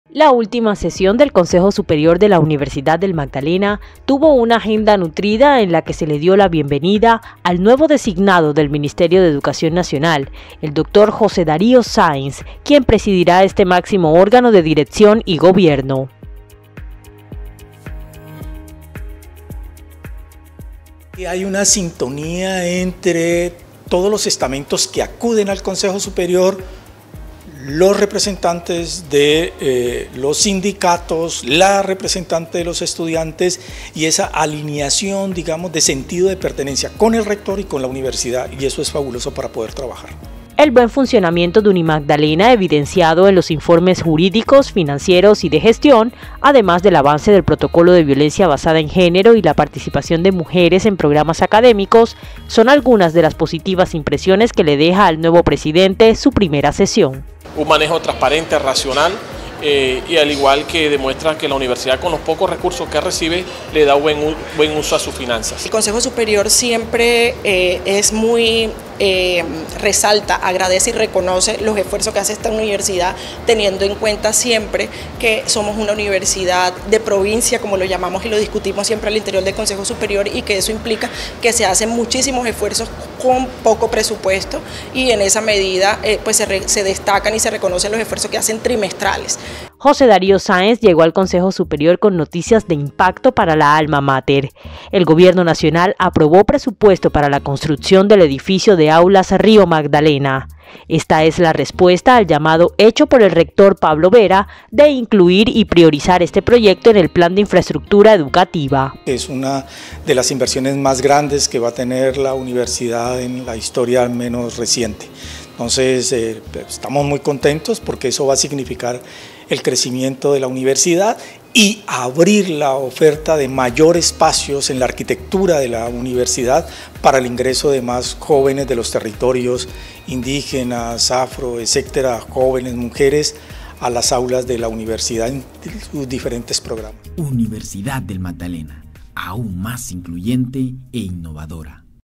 Como presidente del Consejo Superior, asesor de MinEducación destaca buena gestión de la Universidad
Informes de gestión, actividades financieras, buenas noticias desde el ámbito nacional, entre otros asuntos, fueron abordados durante la más reciente sesión del Consejo Superior de la Universidad del Magdalena, en la que además, sus miembros le dieron la bienvenida al nuevo designado del Ministerio de Educación ante el máximo órgano de gobierno y dirección.